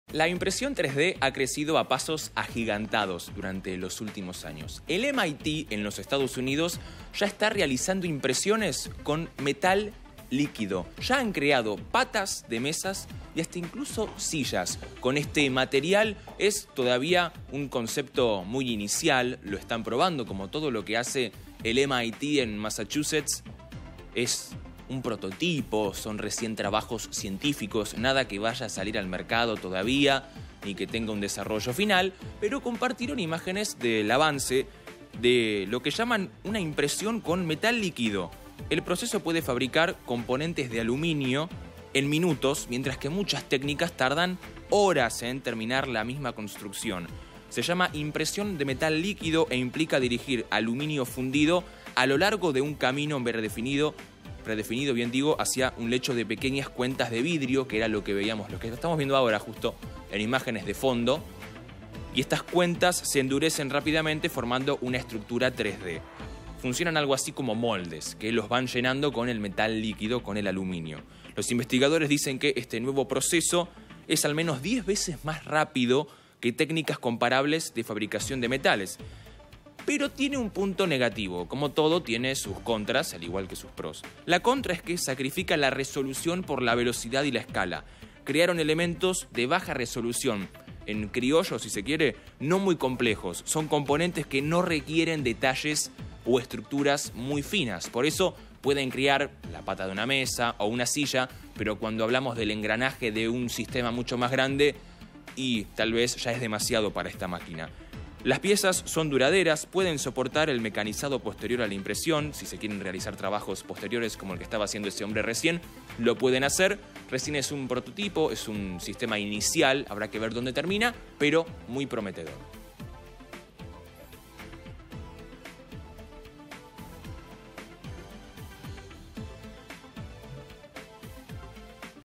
EEUU | Ahora los muebles se pueden "imprimir"
👉 Se llama impresión de metal líquido  e implica dirigir aluminio fundido a lo largo de un camino predefinido hacia un lecho de pequeñas cuentas de vidrio.